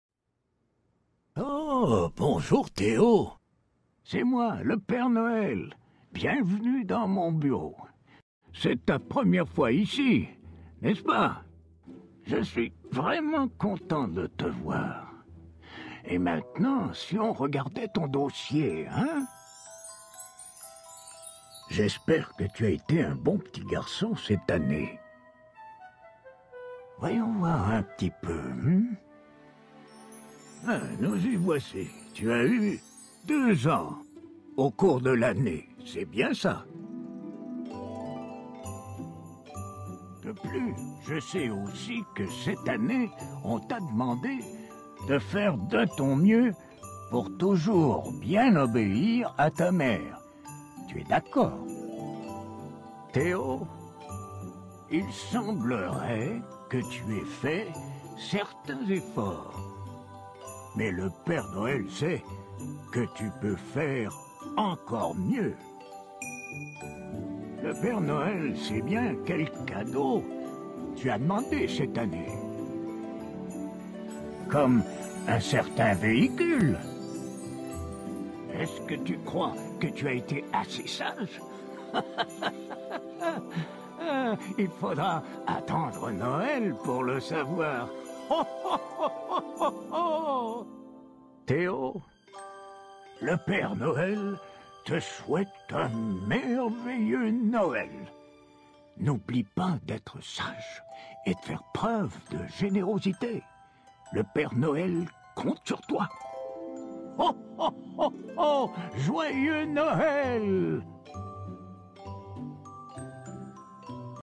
Message de Papa Noël 2010 pour Théo
Message vidéo de Papa Noël, reçu pour Théo afin de lui annoncer l'arrivée future de son tracteur.